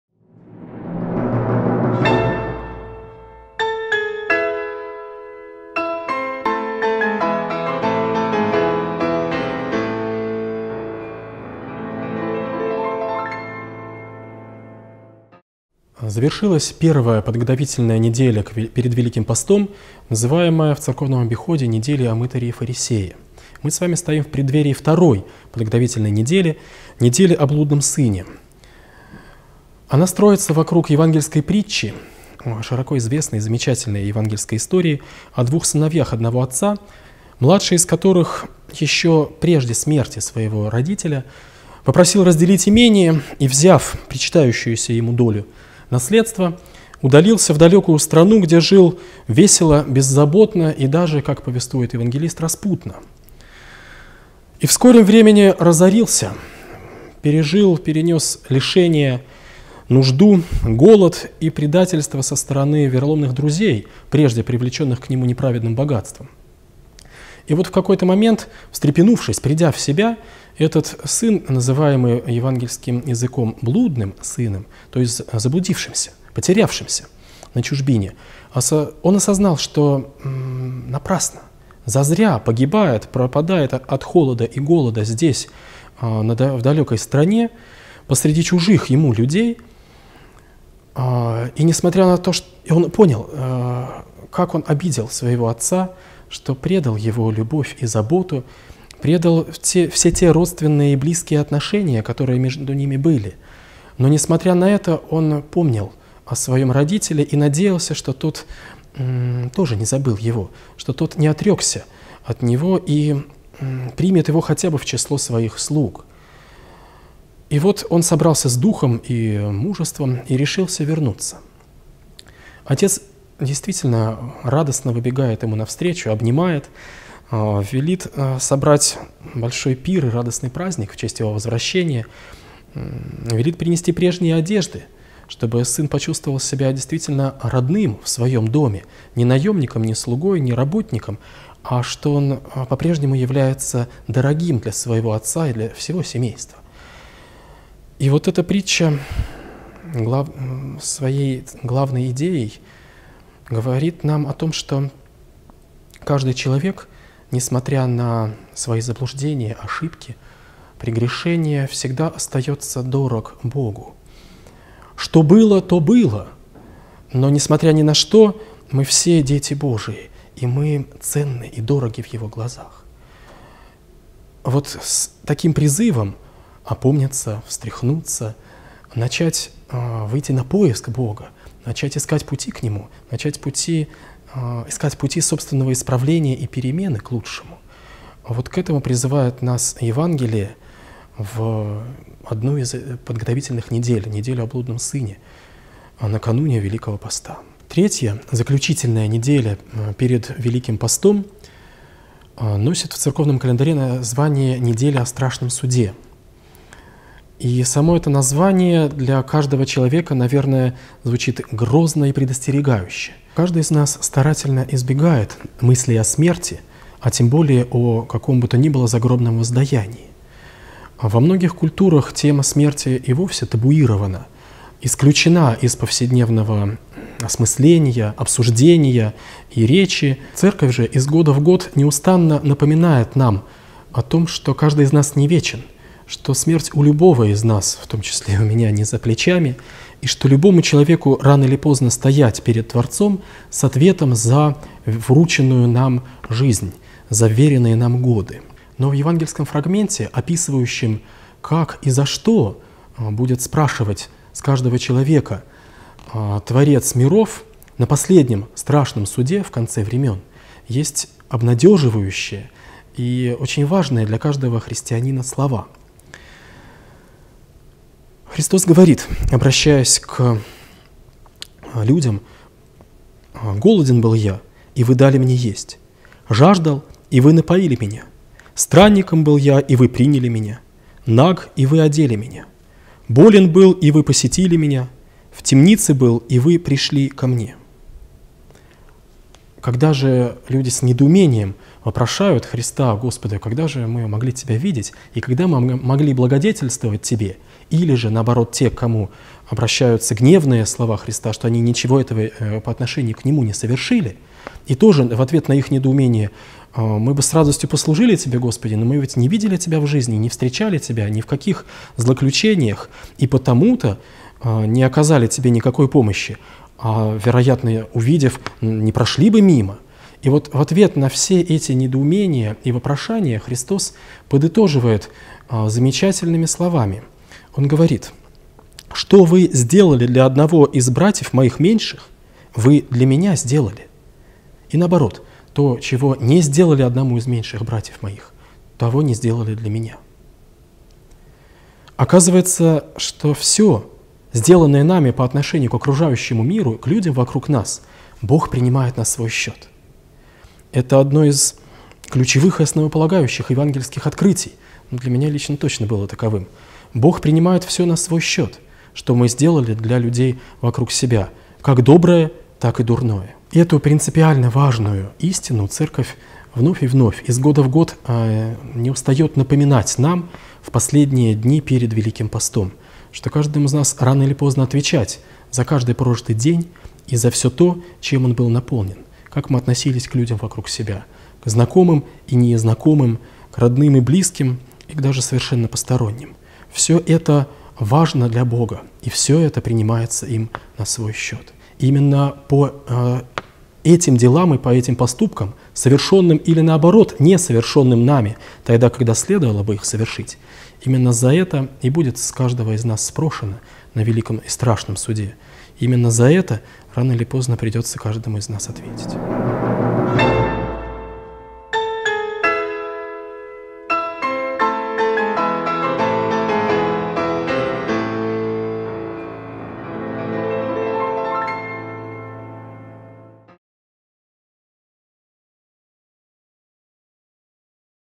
Вопросы о религии. Подготовительные недели перед Великим постом
Перед Великим постом следует несколько Подготовительных недель: о мытаре и фарисее, о блудном сыне, о страшном суде. Как их нужно провести? В чём заключается их смысл?  Отвечает иерей Яков Коробков, клирик Спасо-Вознесенского кафедрального собора г. Ульяновска.